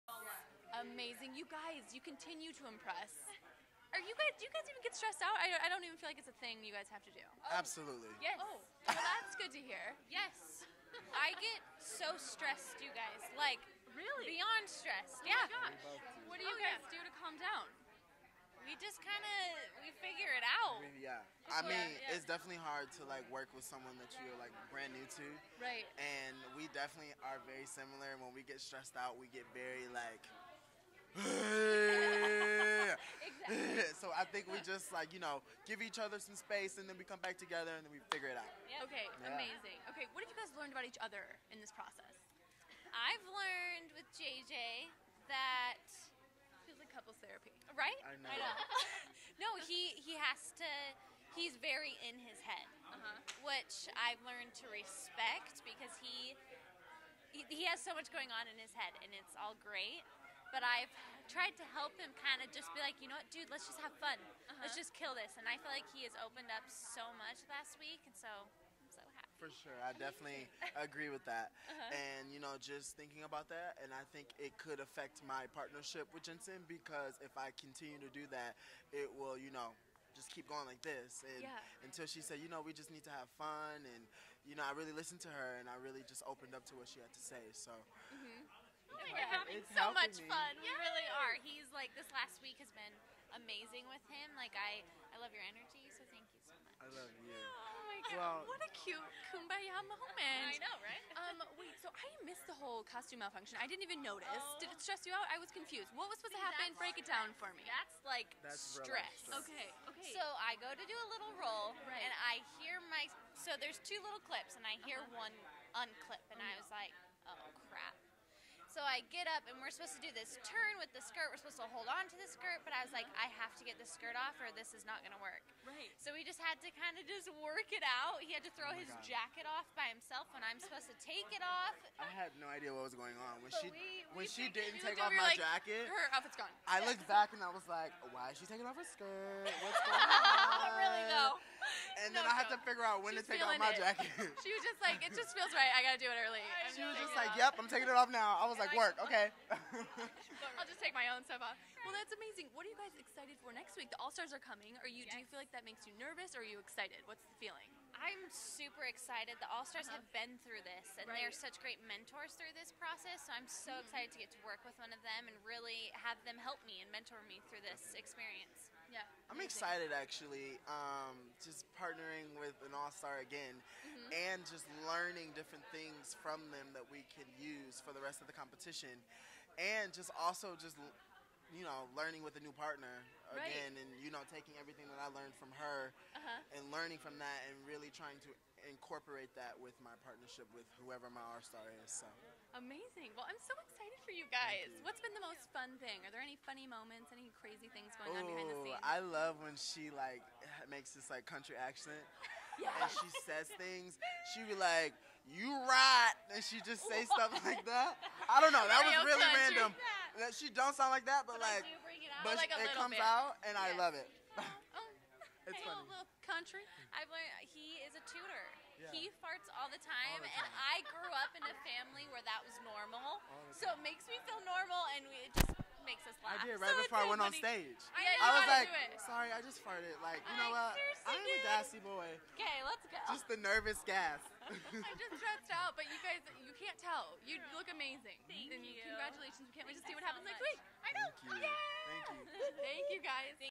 SYTYCD: Jensen Arnold tells ABTV Jay Jay Dixon's secret habit
So You Think You Can Dance contestants Jensen Arnold and Jay Jay Dixon tell ABTV host Tiege Dooley-Panko their favorite things about each other and what they've learned from being on the show!